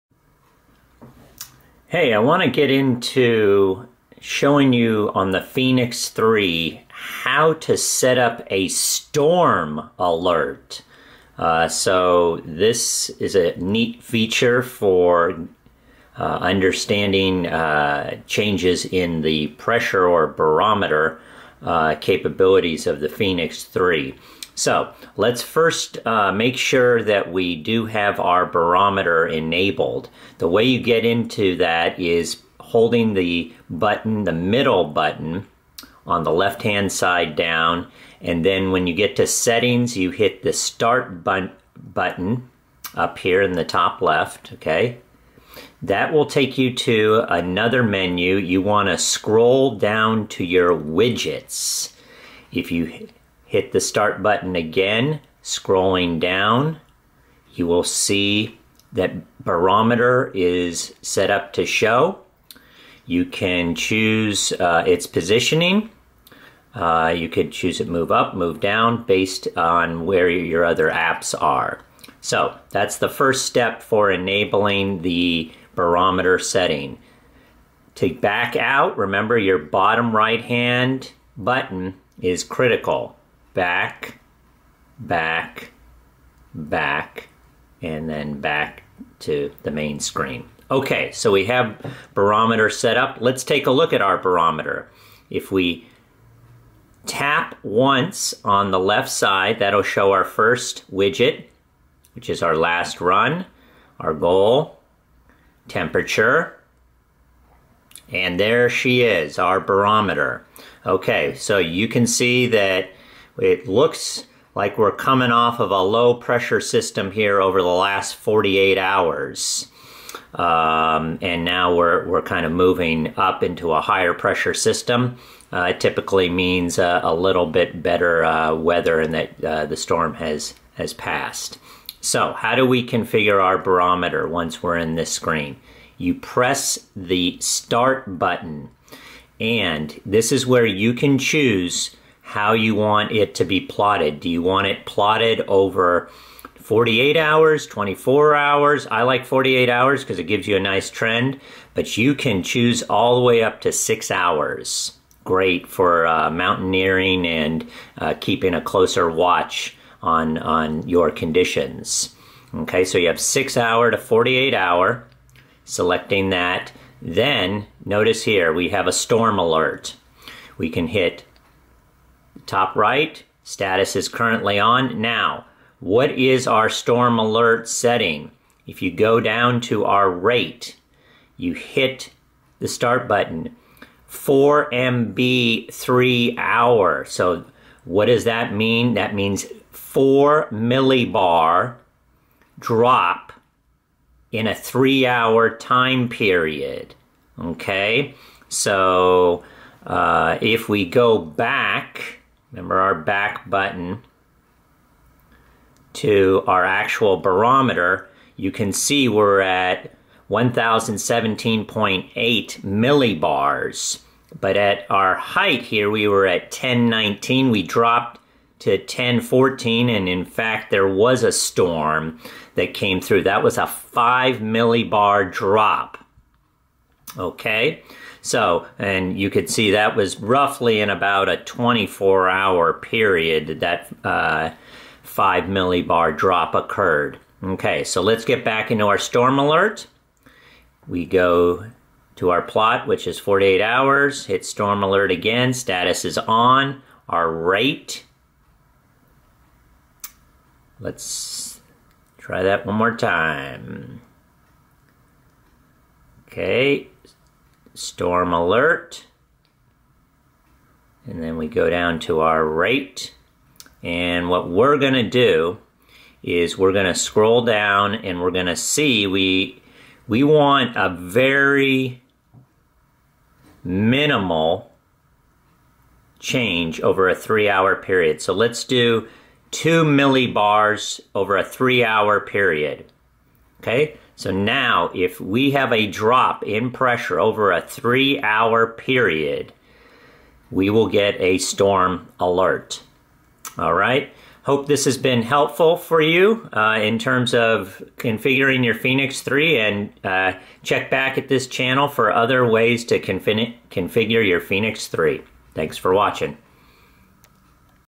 Garmin Fenix3 Storm Alert - Weather - Barometer
How to configure storm alerts for Garmin Fenix3 using the barometer.  Easy to configure the mb per hour and range for drops in the pressure. Configure to track 6 hours - 48 hours.   I personally like to look at 2mb over a 3 hour period.  Measuring pressure changes too can help with health sensitivities .

Purchase this great watch for going on camping or backpacking trips.
Amazon has a good price now with Fenix3 HR just released.